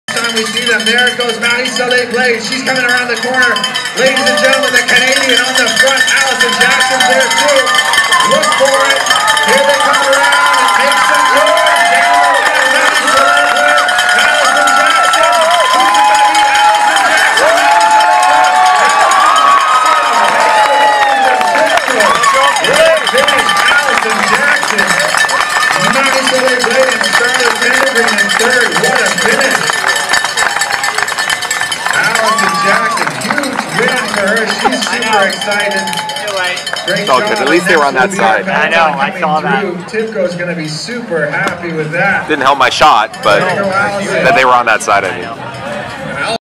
White Spot | Delta Road Race - Women's Finish
1. Alison Jackson (Team TIBCO-Silicon Valley Bank) - Vermillion, AB
2. Marie-Soleil Blais (Astana Women's Team) - Montreal, QC
3. Starla Teddergreen (Hagens Berman - Supermint) - Portland, OR